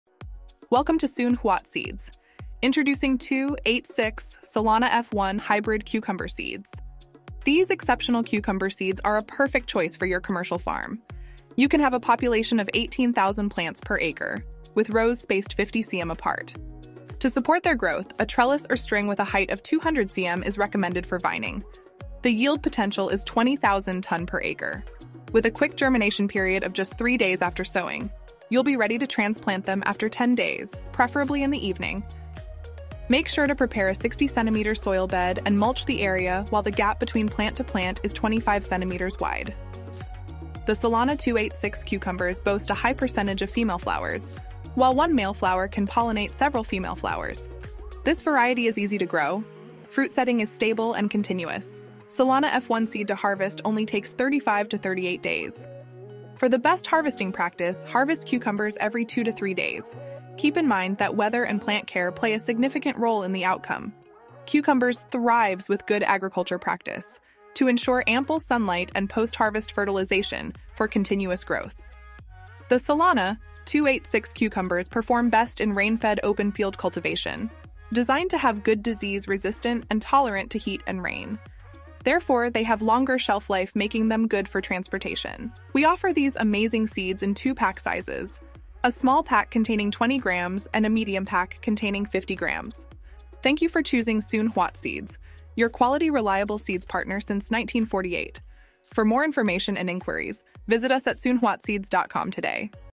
Solana 286 F1 | 300-500g per fruit | Seed to Harvest 35 Days | 18-25 Ton/Acre | Disease Resistance
- Average Fruit Weight: 300g - 500g per cob
- Days to Harvest: 35 - 38 days
- Size: [L] 18-22cm x [W] 5-6cm
- Medium to Dark Green
- Resistant to heat & rain
- Able to continue produce flowers and fruit evently

Get Your Seeds Now at our Soon Huat Seeds Apps.

Download our Soon Huat Seeds Apps Here and Get Access to Our Seeds Easily!